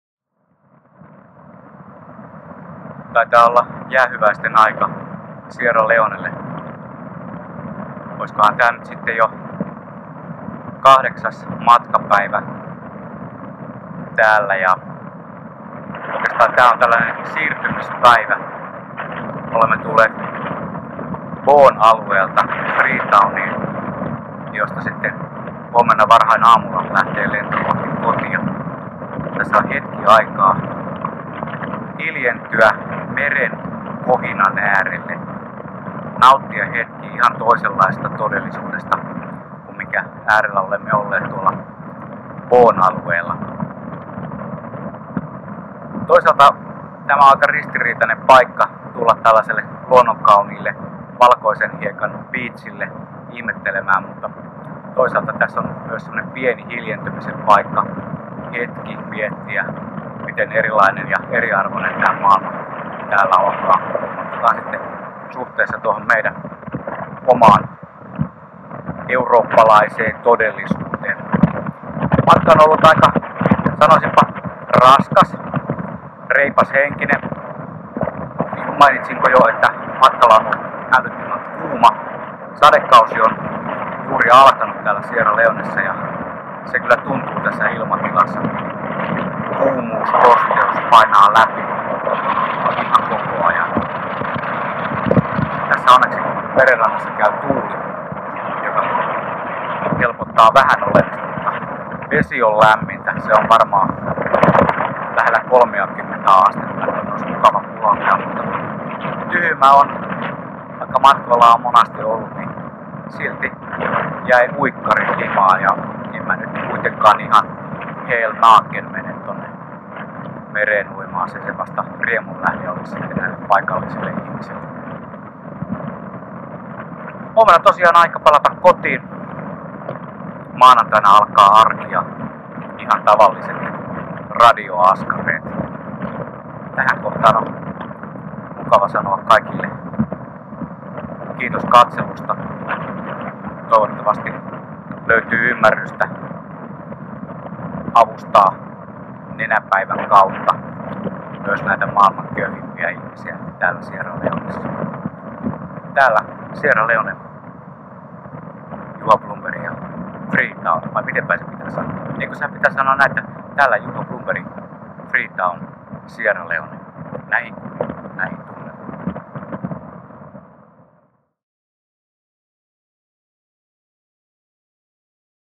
Blomis Sierra Leonessa osa 8
Juha Blomberg Nenäpäivämatkalla Sierra Leonessa toukokuussa 2014. Videopäiväkirja - viimeinen päivä.
(pahoittelemme äänen huonoa laatua videossa)